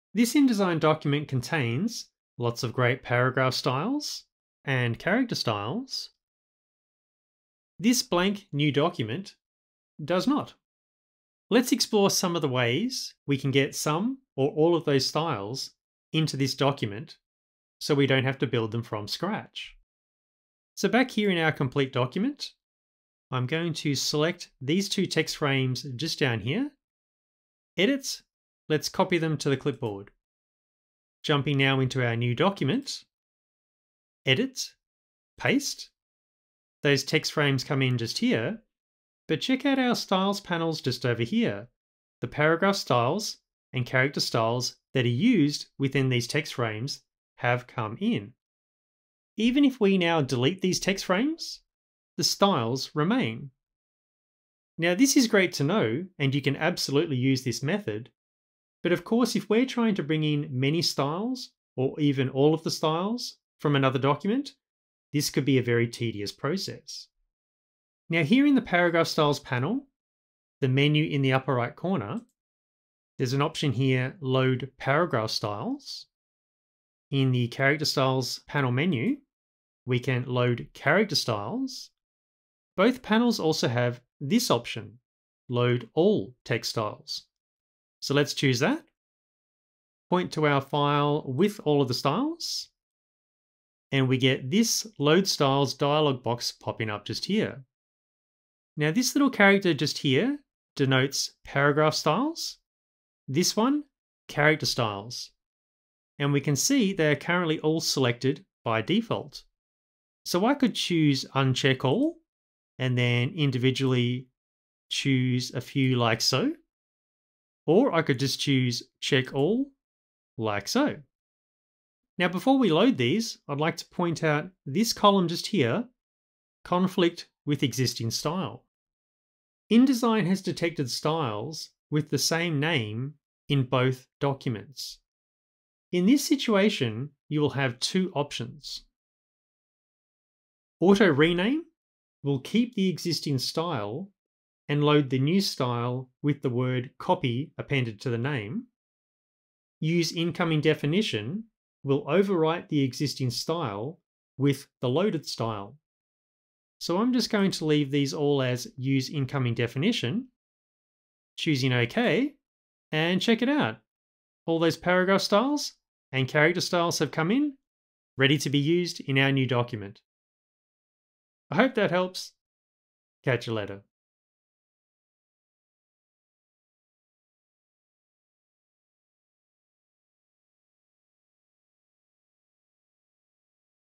Load Paragraph and Character Styles From Another InDesign File
Learn how to get some, or all, of the styles from one InDesign document into another. Details below...

0:00 - Copy Styles Between Documents
Having copied text to the clipboard that has applied Paragraph Styles and/or Character Styles, pasting that text into another document will bring in those styles. Even if the text is immediately deleted, the styles will remain. This is a fantastic way to transfer a few styles but could be very time consuming if wanting to transfer many, or all, styles to another document.

1:07 - Load Styles From Another Document
In the upper right corner of both the Paragraph and Character Styles panels is a menu that contains the option "Load All Text Styles". Within the dialog box...

Select Styles:
Next to each style is a checkbox for inclusion during import. These can all be simultaneously changed via the "Check All" and "Uncheck All" buttons.

Style Conflicts:
If InDesign detects a style with the same name in both documents, two choices can be made for each style...
1. Auto-Rename will keep the existing style and load the new style with the word copy appended to the name.
2. Use Incoming Definition will overwrite the existing style with the loaded style.